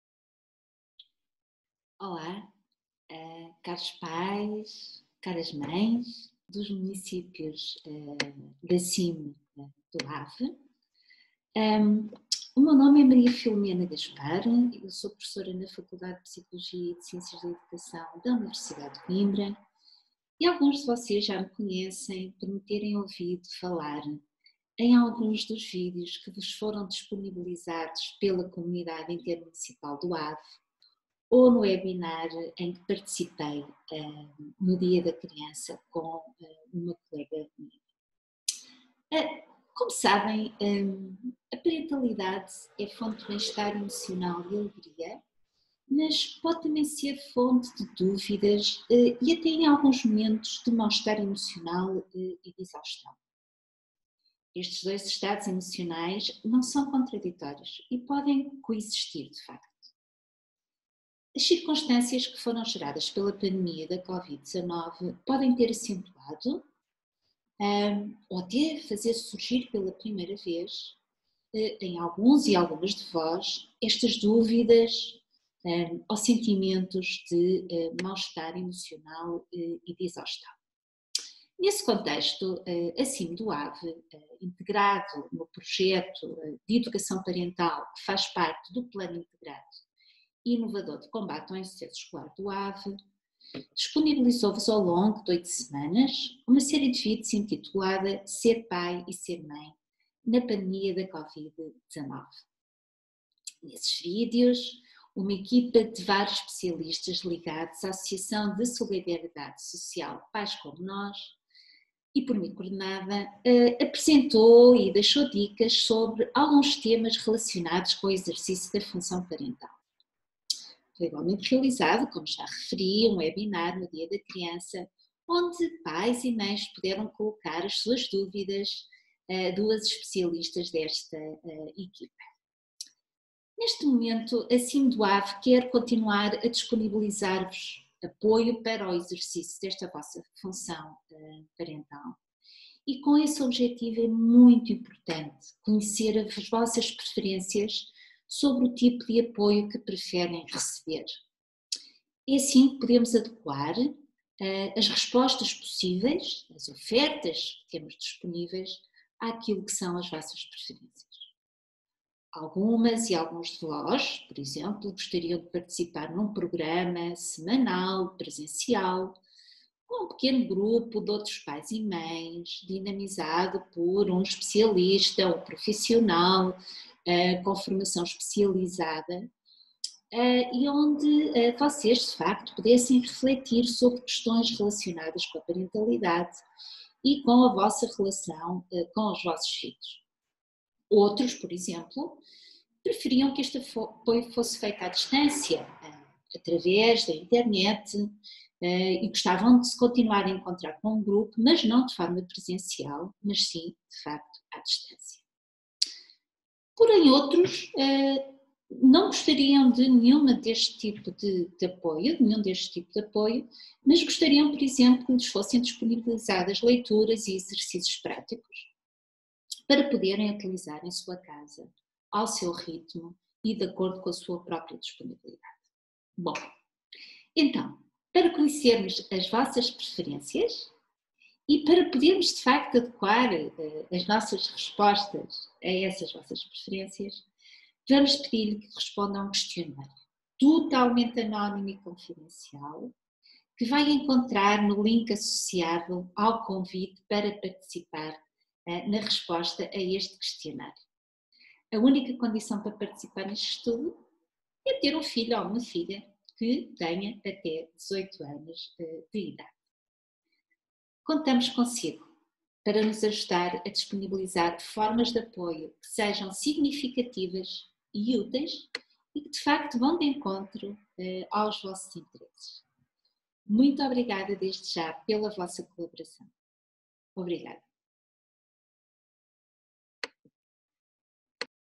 Desafios à parentalidade: Enquadramento para um questionário de preferências parentais
Ser Mãe e Ser Pai na pandemia da Covid-19 - Projeto de Educação Parental do PIICIE do Ave.

Caras mães e caros pais, digam-nos as vossas preferências sobre diferentes formas de  apoio ao desempenho da vossa função parental, respondendo a um questionário disponível em: